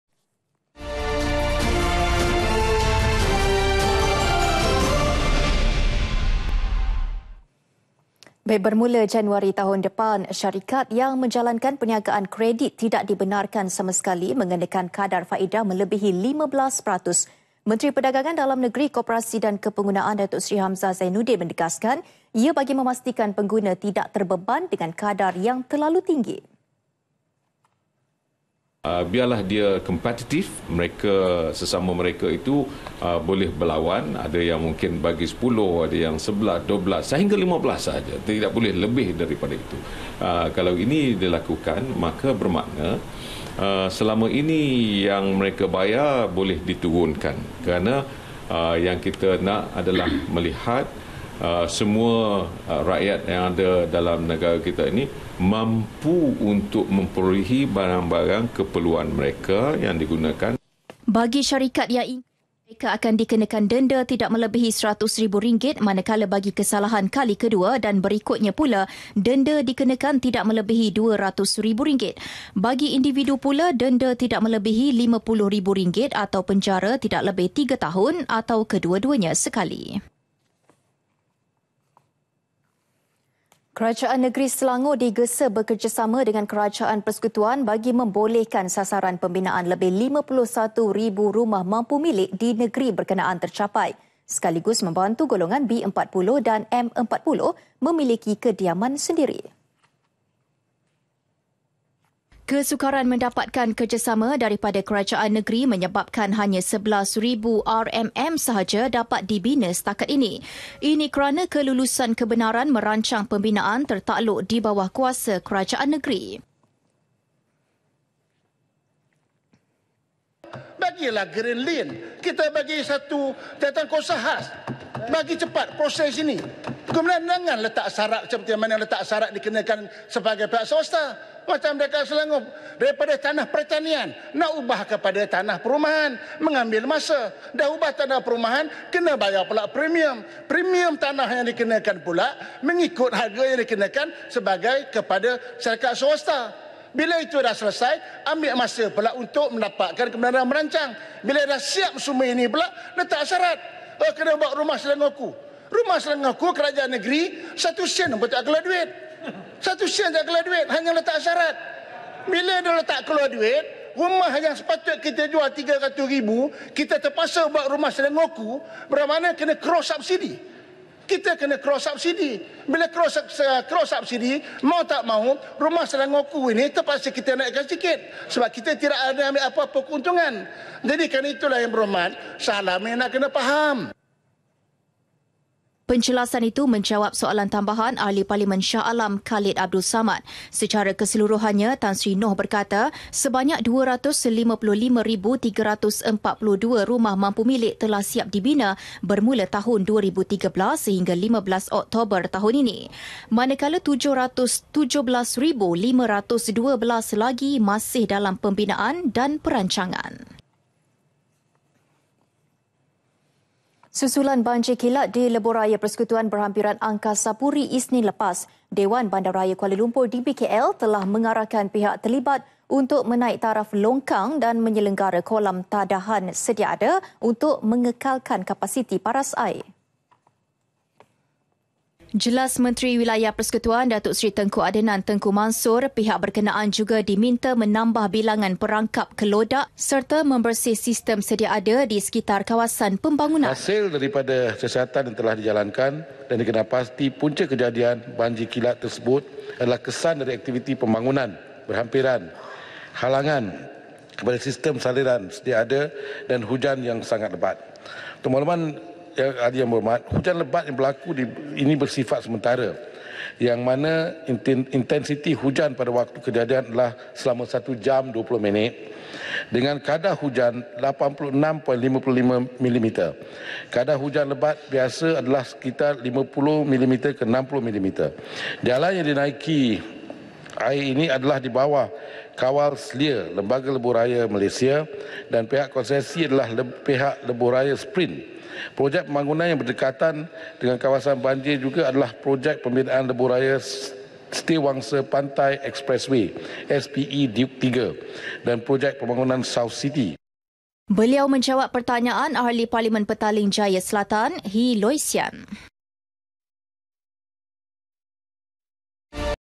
LAPORAN PERSIDANGAN PARLIMEN [2 NOV 2017]
I. Bermula Januari tahun hadapan, syarikat yang menjalankan perniagaan kredit tidak dibenarkan sama sekali mengenakan kadar faedah melebihi 15 peratus.

II. Malaysia tidak menerima sebarang reaksi negatif daripada Arab Saudi, Bahrain, Mesir dan Emiriyah Arab Bersatu - UAE , susulan lawatan Emir Qatar - Sheikh Tamim Hamad Al Thani ke negara ini, bulan lepas.

III. Kerajaan Negeri Selangor digesa bekerjasama dengan kerajaan persekutuan bagi membolehkan sasaran pembinaan lebih 51 ribu rumah mampu milik di negeri berkenaan tercapai.

IV. Kerajaan Negeri Selangor digesa bekerjasama dengan kerajaan persekutuan bagi membolehkan sasaran pembinaan lebih 51 ribu rumah mampu milik di negeri berkenaan tercapai.

V. Susulan banjir kilat di Lebuh Raya Persekutuan berhampiran Angkasapuri, Isnin lepas.

Dewan Bandaraya Kuala Lumpur DBKL telah mengarahkan pihak terlibat untuk menaik taraf longkang dan menyelenggara kolam tahanan sedia ada untuk mengekalkan kapasiti paras air.